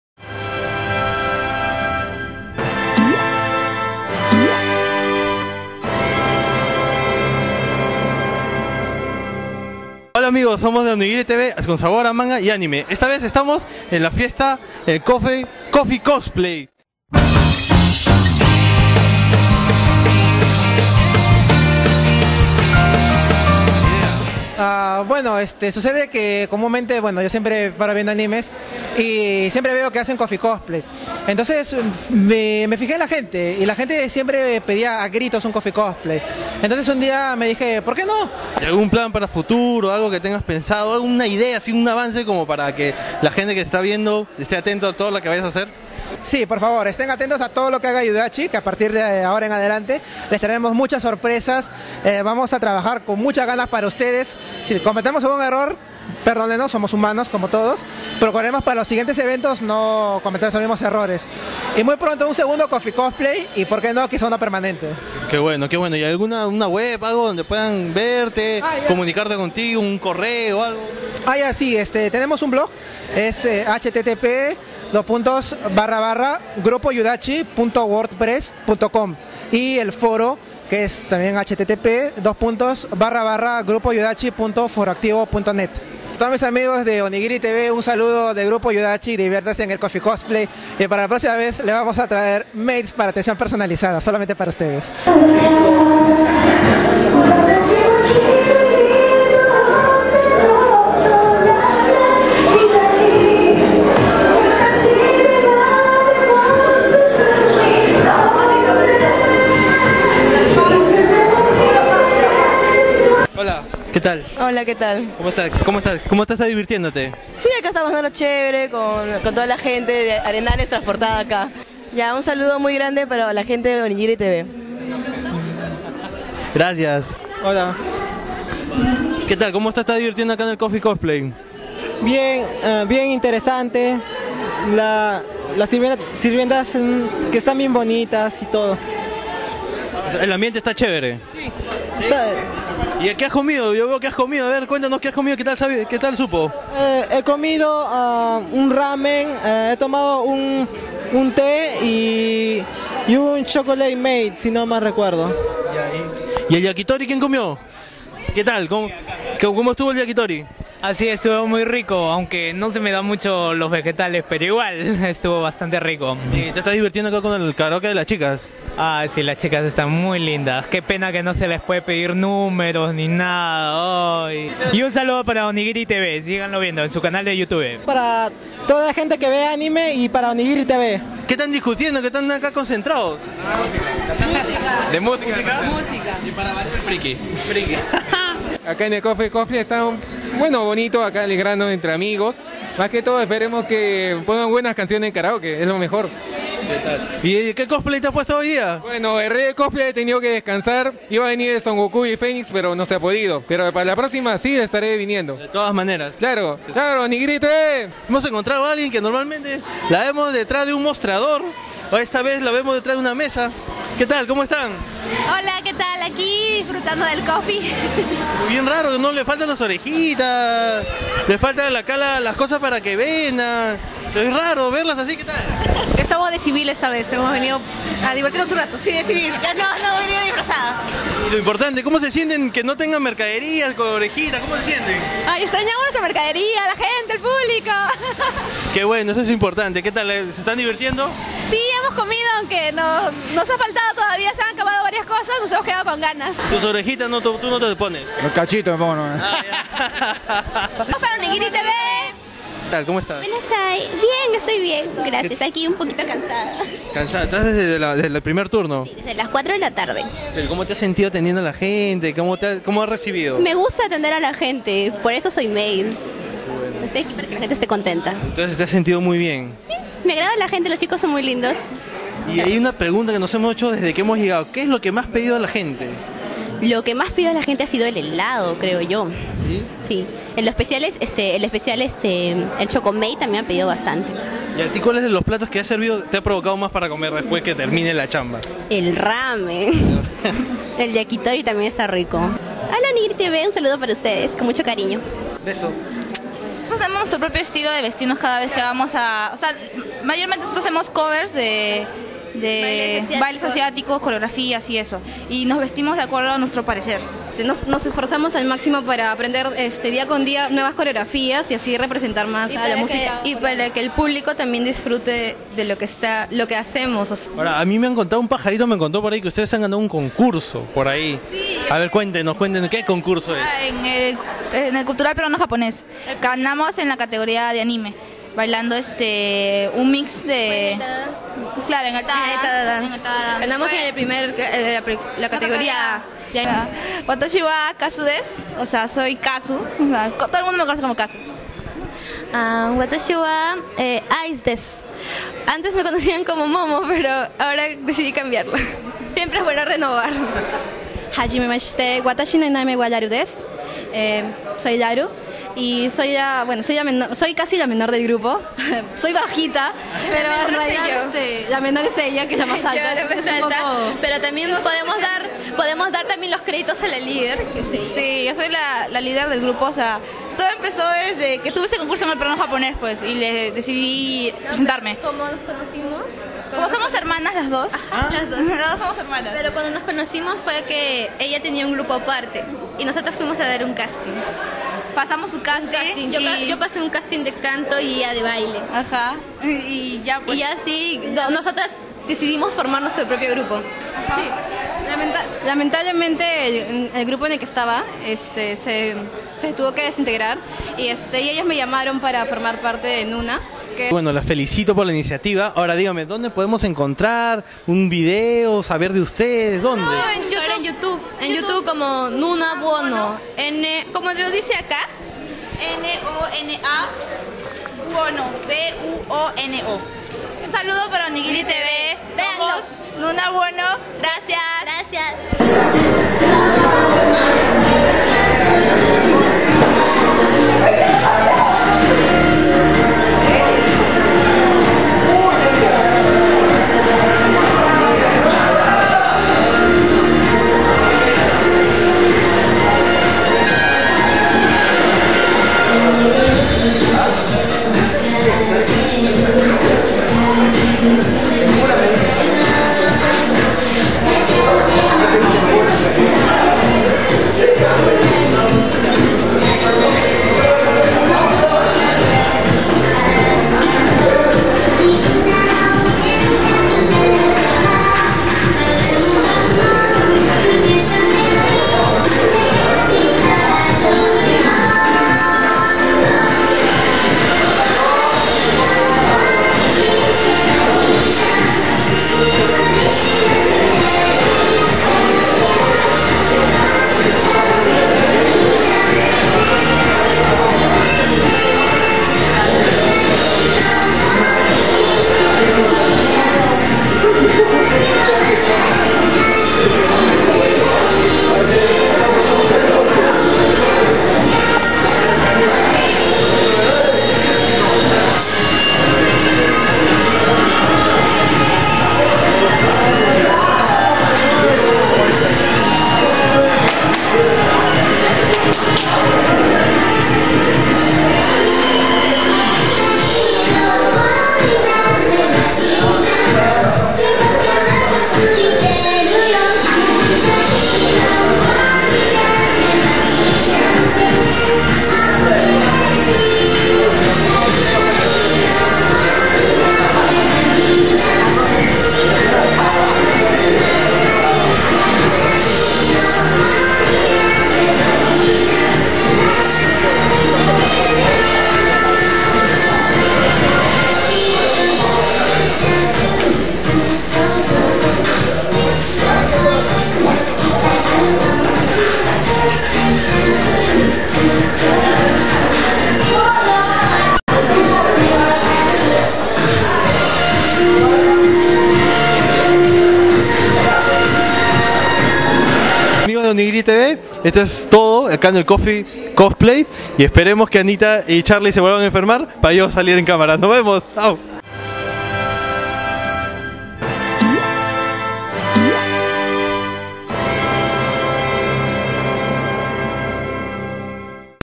Coffee Cosplay Yuudachi.
1er Coffe Cosplay organizado por el grupo Yuudachi, el dia 9 de Enero. onigiritv llevo sus cámaras al evento para conocer que hay de nuevo...